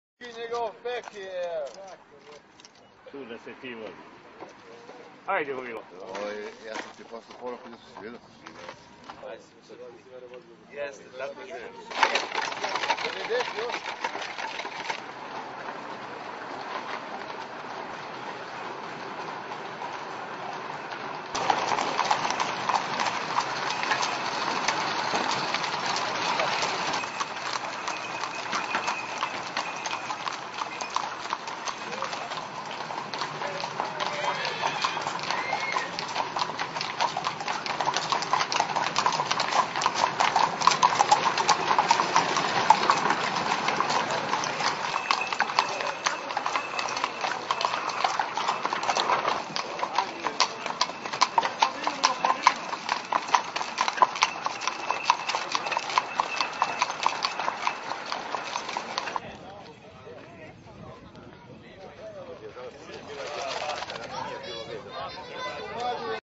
kg fijakerijada pok 0701
Druga božićna fijakerijada u Kragujevcu